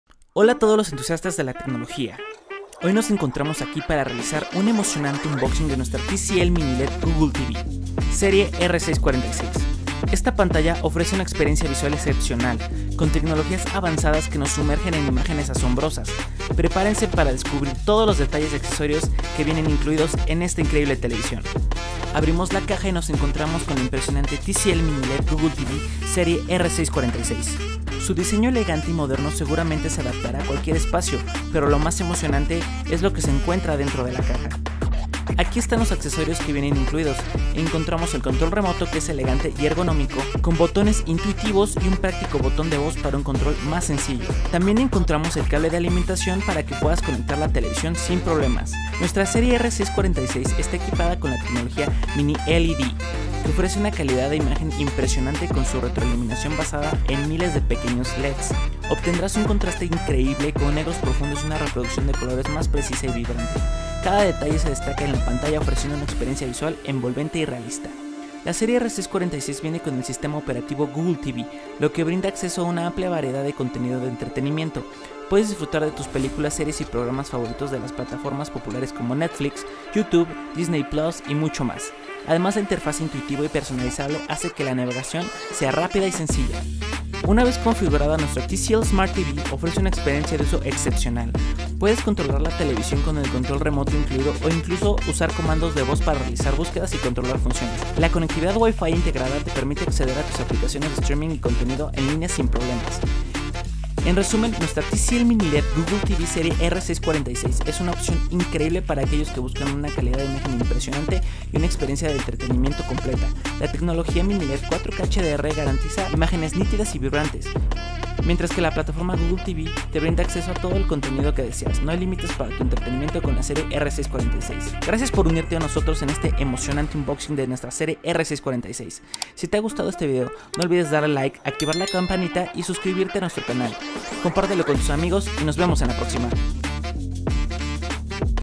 TCL MINILED GOOGLE TV, serie R646 | Unboxing
Conoce una de nuestra series consentidas y favoritas ¡TCL MiniLED Google TV!
Este televisor cuenta con todo lo que necesitas para disfrutar de cada detalle de tu película favorita gracias a su tecnología MiniLED, resolución 4K QLED, Google Assistant con Hands Free Voice Control ¡Y mucho más!